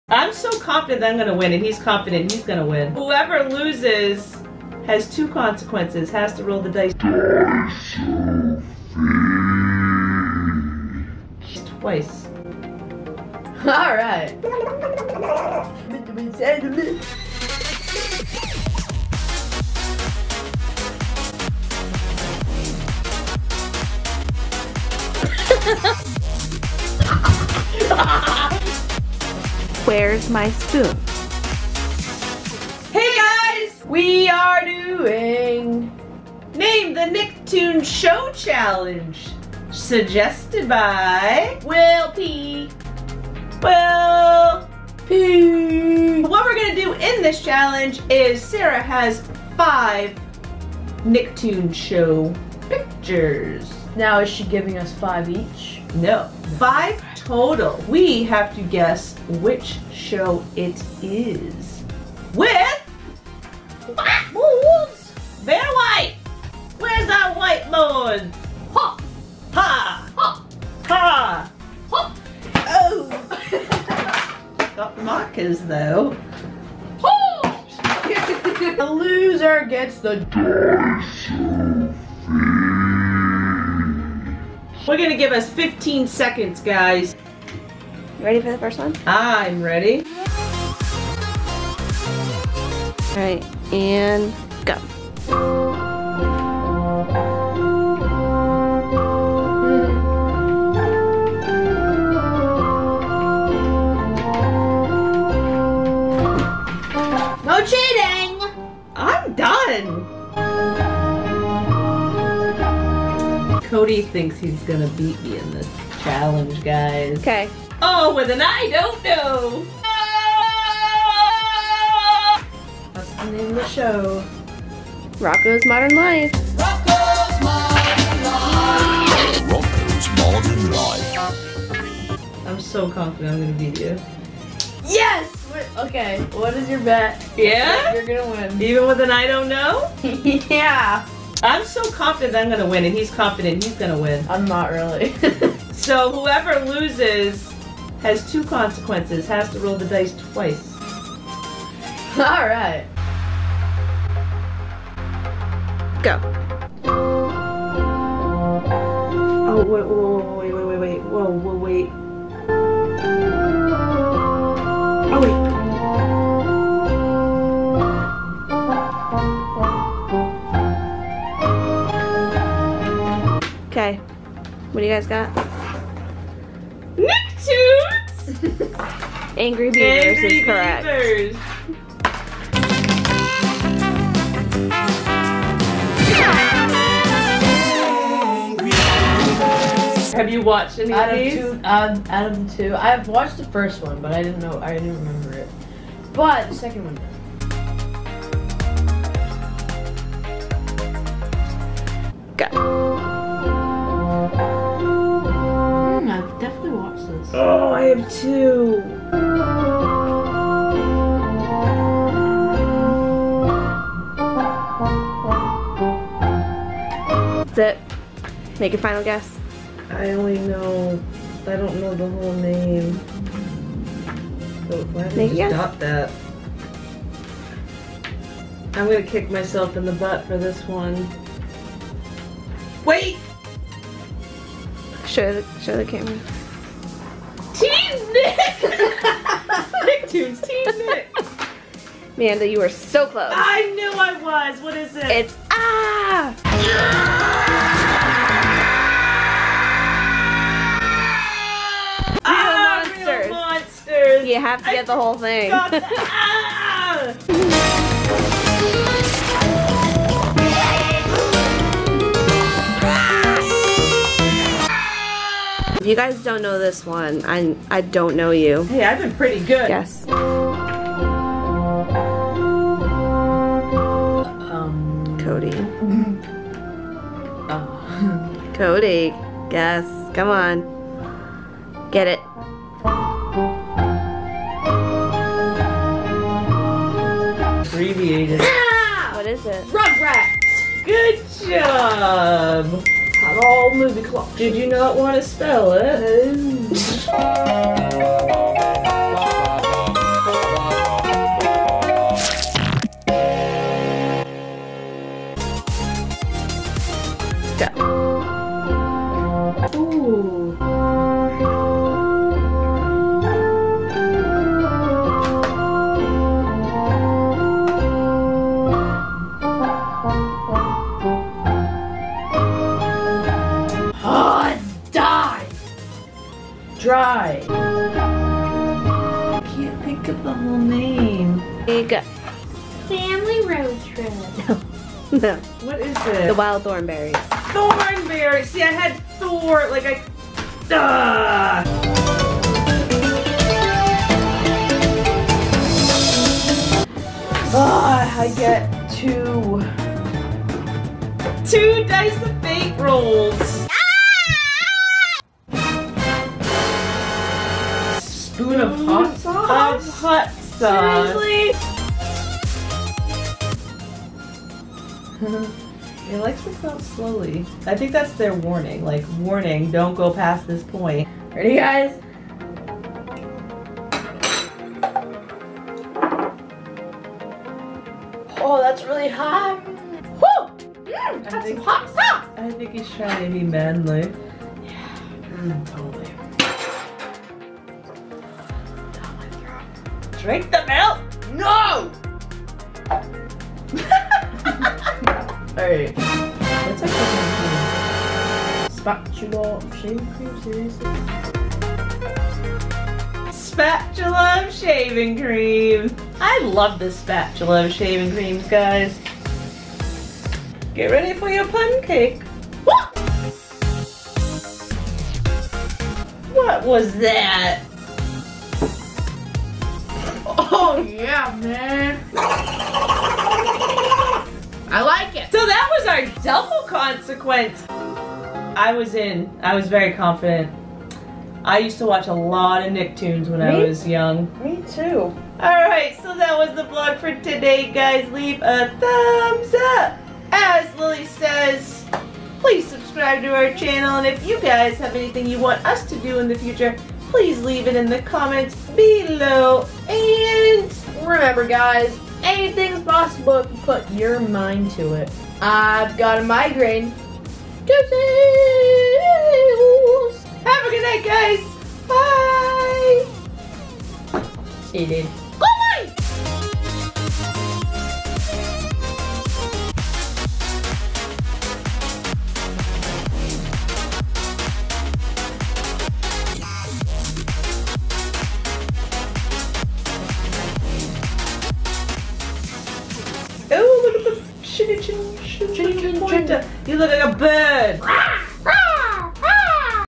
NAME THE NICKTOONS SHOW CHALLENGE! (The Loser gets 2x's the "DICE OF FATE!")
Cody and I did the "NAME THE NICKTOONS SHOW CHALLENGE" Suggested by, Will P. In this challenge... Sarah gave us 5 Nicktoon show pictures and we had to guess the show title. With Cody and I both feeling very confident with this challenge... We decided that whoever get's the most wrong... Has 2x's the "DICE OF FATE" roll. Things get crazy! Who will it be? Watch and see... ENJOY!